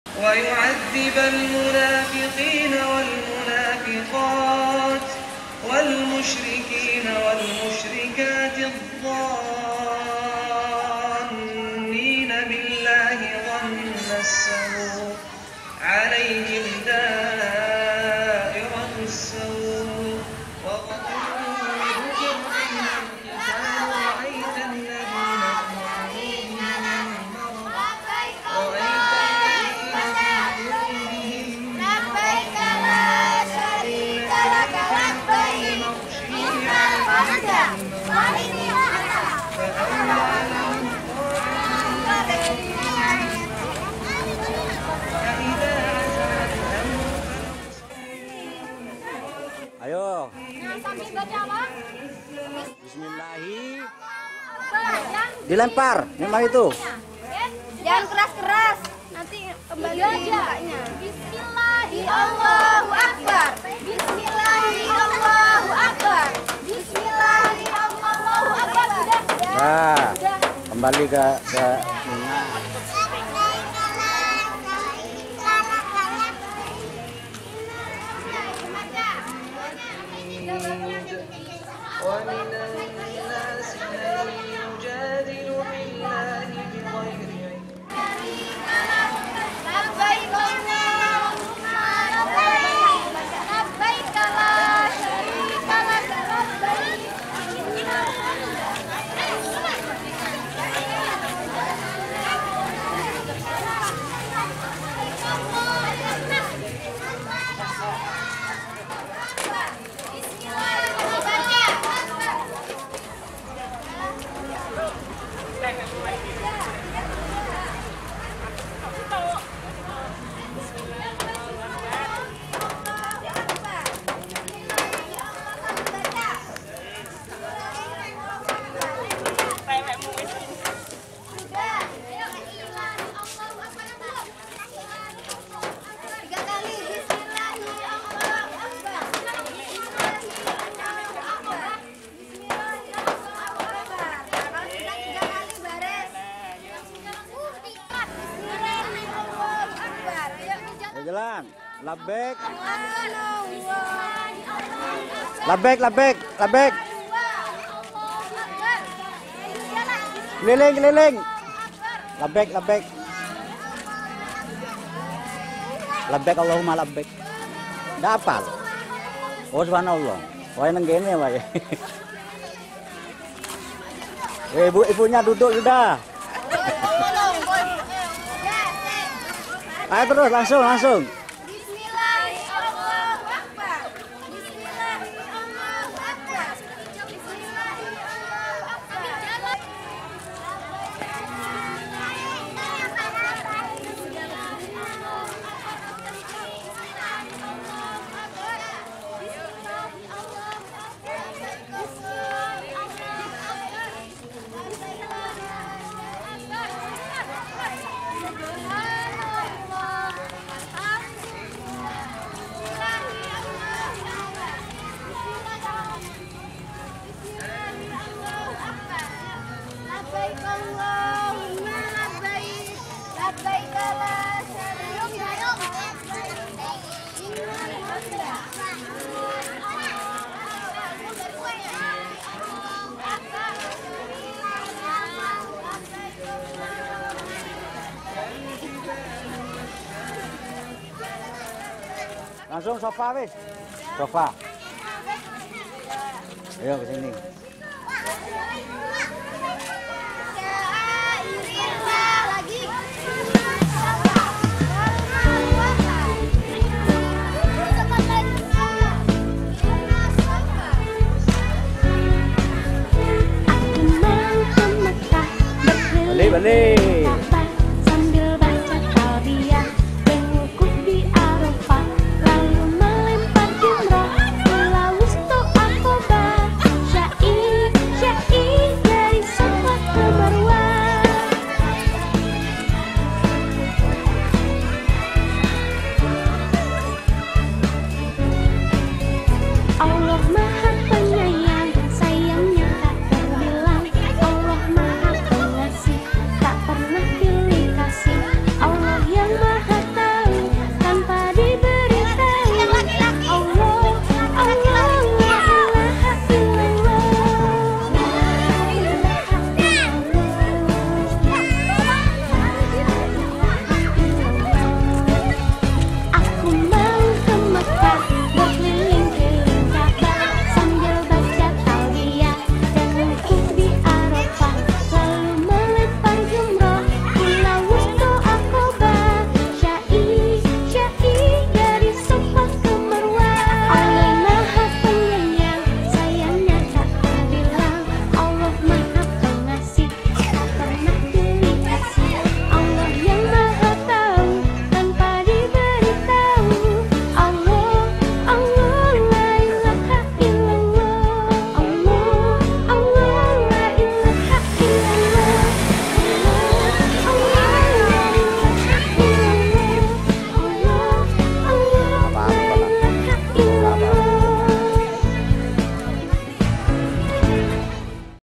Simulasi Manasik Haji R.A Ar Raihan
dengan tujuan untuk menumbuhkan cita-cita melaksanakan rukun islam yang ke 5 maka RA. Ar Raihan mengadakan simulasi manasik haji didepan kelas dengan harapan para murid kenal dengan istilah-istilah dan simbol-simbol dalam agama seperti ka'bah, maqom ibrahim, tawaf, sa'i, lempar jumroh dll sehingga diharapkan tumbuh keinginan untuk berziarah ke baitullah. selain dari itu sebagai pengetahuan baru bagi para murid dan orang tua murid